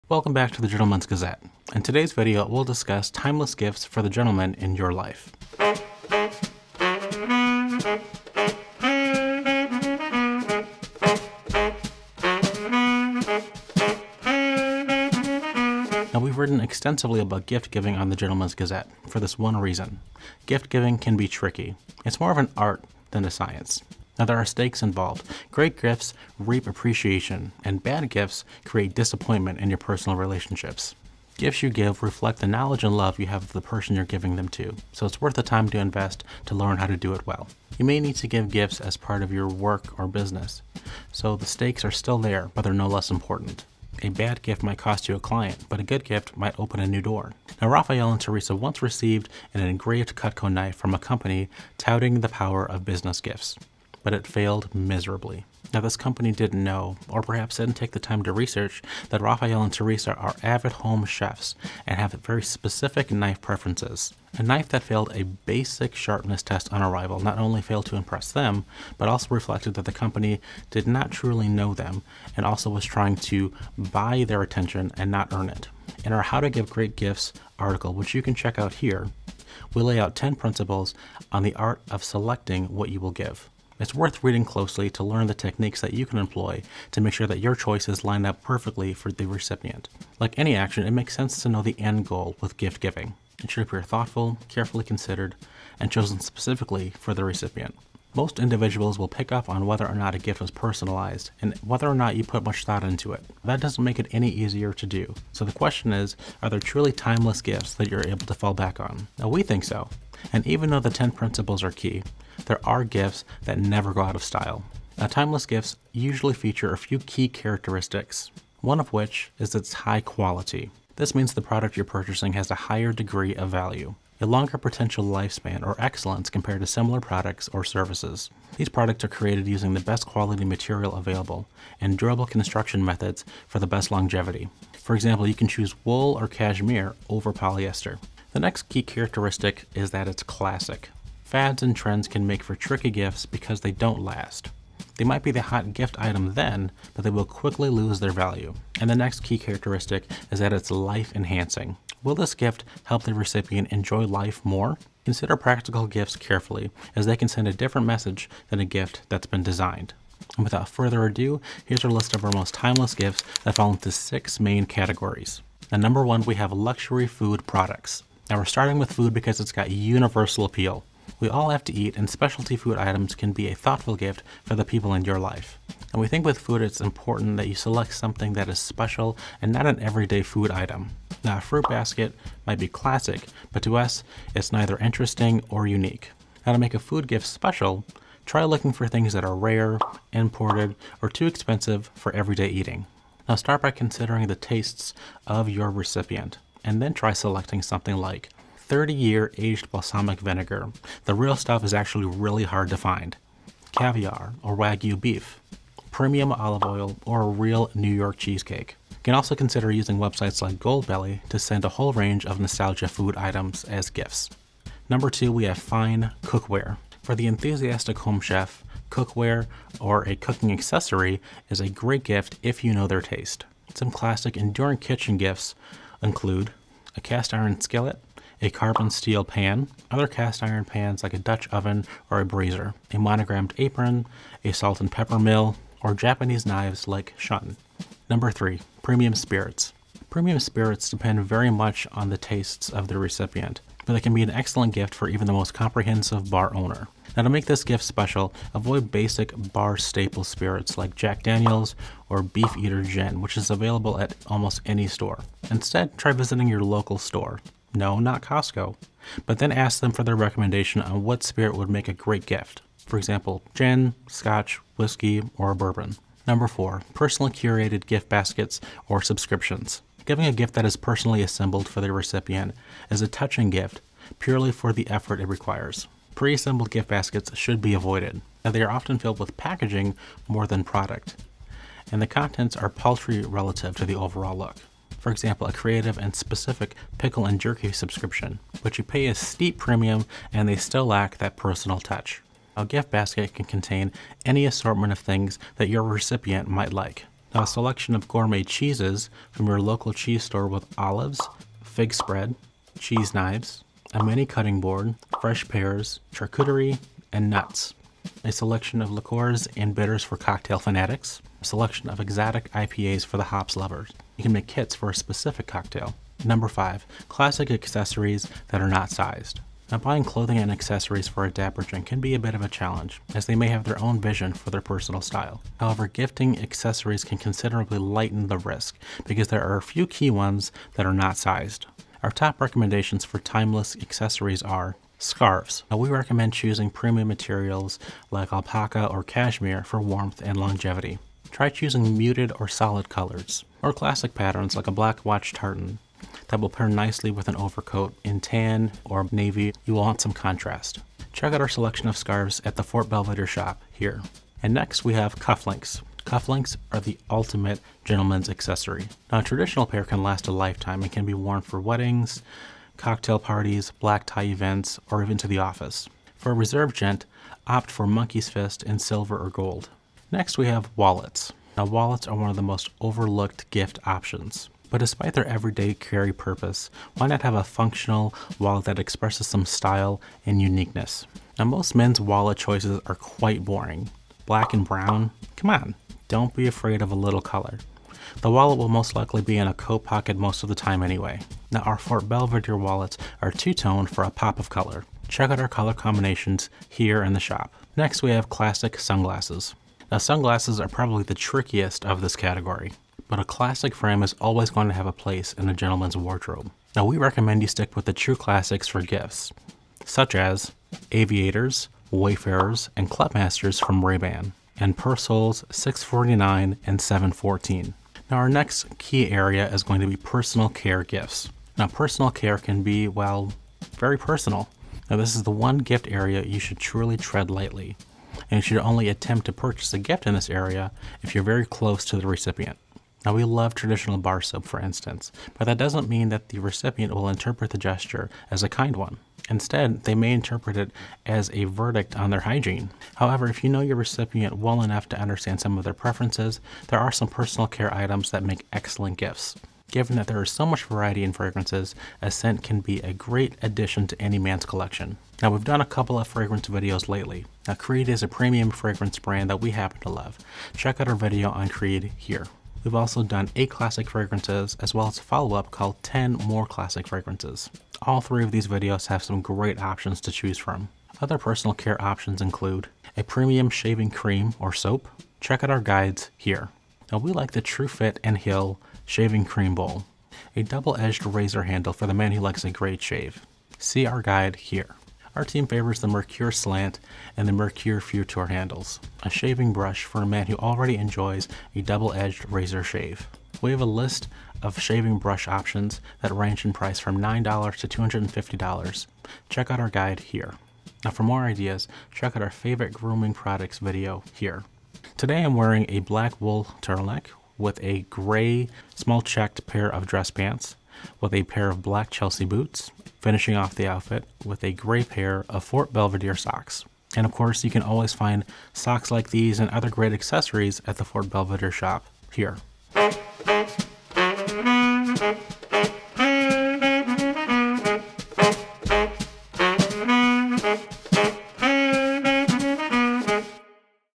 6 Timeless Gifts for Gentlemen - Gift Ideas for Christmas & More Holidays
We round up 6 timeless gift ideas perfect for the gentlemen in your life:

VIDEO CREDITS:
→ Script: Teresa C. Schneider
→ Camera: Chris Dummer
→ Editing: Jonathan Oster

00:00 Introduction

Gifts you give reflect the knowledge and love you have the person you’re giving them to so it’s worth the time to invest to learn how to do it well. You may need to give gifts as part of your work or business so the stakes are still there but they’re no less important. A bad gift might cost you a client but a good gift might open a new door.

Like any action, it makes sense to know the end goal with gift-giving: ensure if you’re thoughtful, carefully considered, and chosen specifically for the recipient. Most individuals will pick up on whether or not a gift was personalized and whether or not you put much thought into it. That doesn’t make it any easier to do!

01: 58 Characteristics of Timeless Gifts
1. It’s of High Quality
2. It’s Classic
3. It’s Life-Enhancing

6 Timeless Gifts for Gentlemen
02:47 1. Luxury Food Products
03:36 2. Fine Cookware
03:59 3. Premium Spirits
04:27 4. Personally Curated Gift Baskets
05:17 5. Classic Accessories (That Aren’t Sized)
           * 05:35 Scarves
           * 05:55 Cufflinks
           * 06:11 Wallets
           * 06:39 Classic Sunglasses
07:01 6. Personal Care Gifts
           * 07:30 Fragrances
           * 07:54 Shaving Cream or Soap

Thoughtful gift-giving is an art that a gentleman acknowledges as important, and timeless gifts are a good place to start. Not only will it make the recipient feel special, but it will also nurture your relationships.

08:25 Outfit Rundown